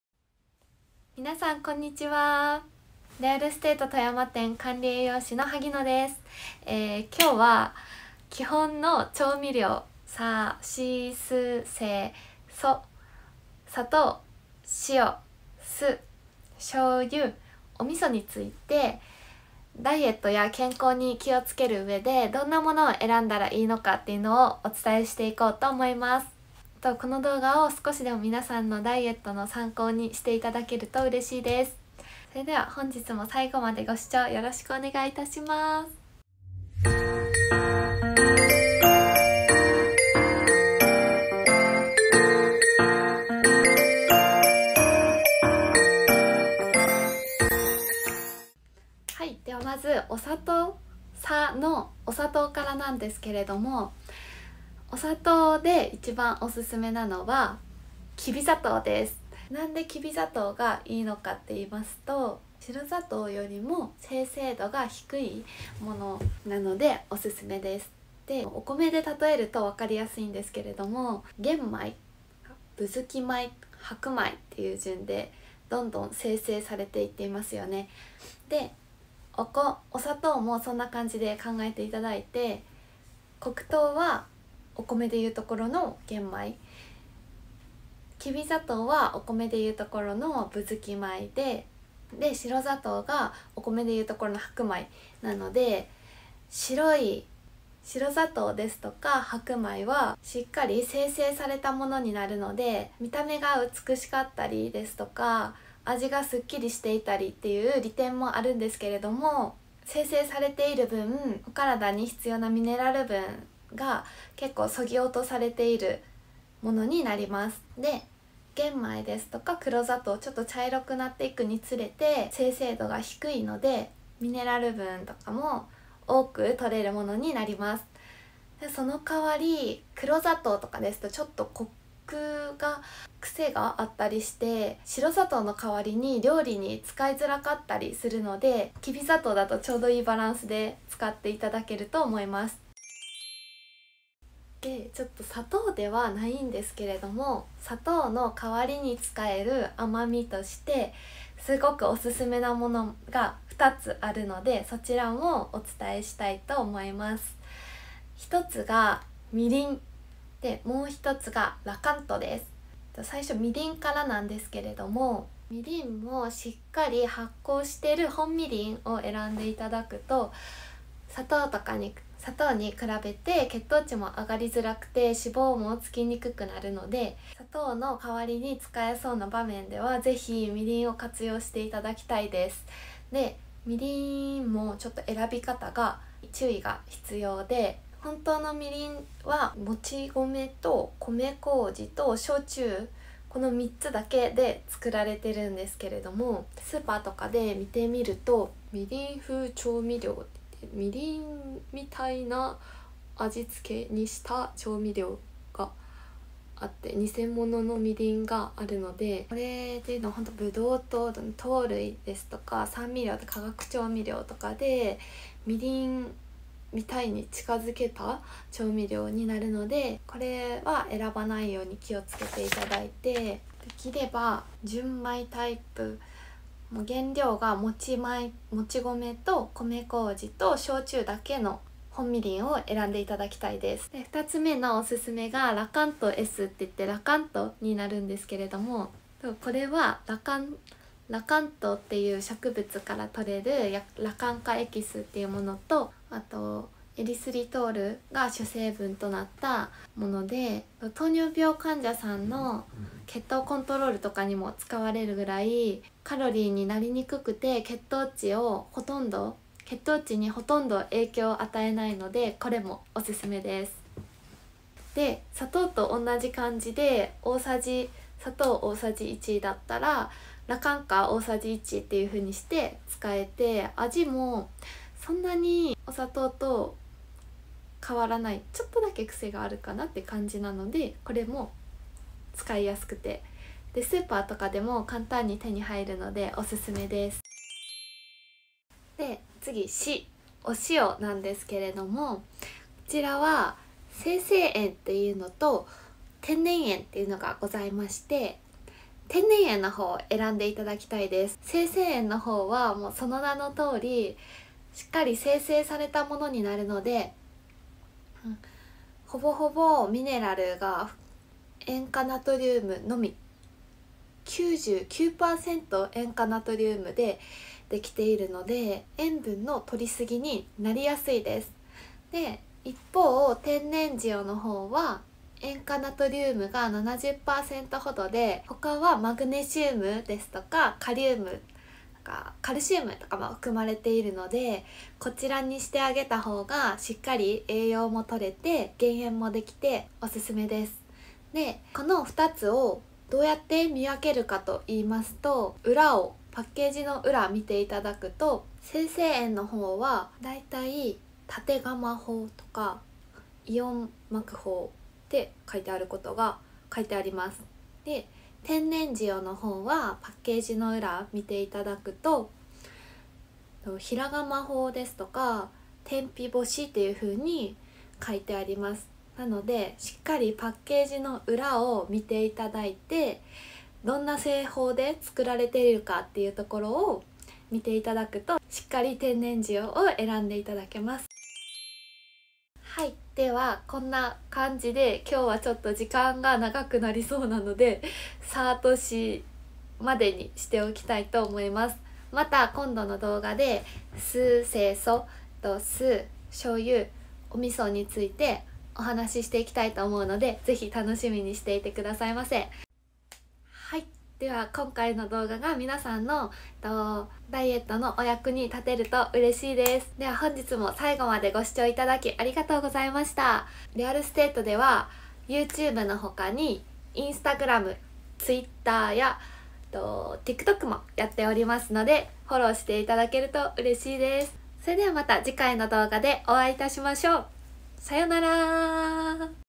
【調味料の選び方】【砂糖、塩編】
日々、お料理に使う調味料、適当に選んでいませんか？？
調味料を見直すと、ダイエットや健康作りがもっと楽しくなります🥰
この動画を参考に、日々使う調味料を出来るところから変えて行って頂けると嬉しいです❤️

◆Instagramも更新中！是非ご覧下さいませ😊❤️